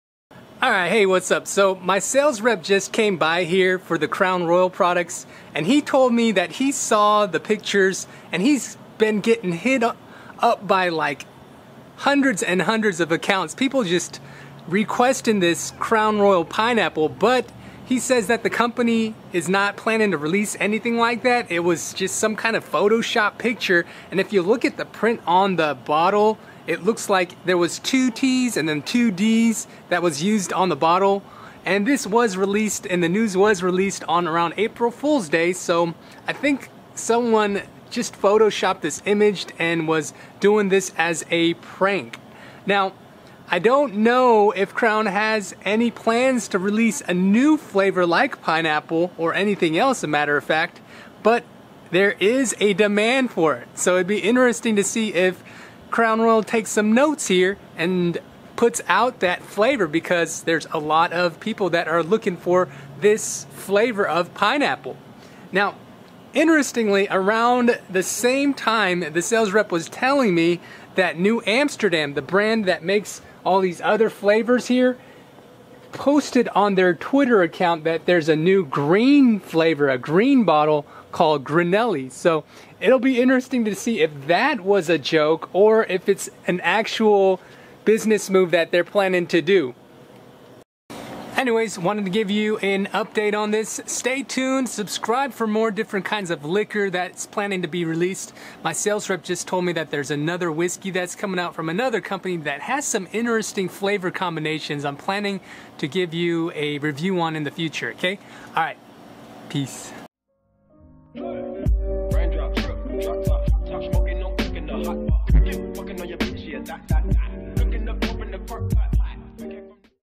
Crown Royal Pineapple Update!
Just talked with the sales rep, looks like there is no such Crown Royal Pineapple that is being released this summer.  Hopefully someone from Crown Royal is catching onto the hype of it being a possibility though.  What do you think, would it sell?

The promescent spray is a great way to delay ejaculation and is easy to use.  3 sprays helped to give me peak performance.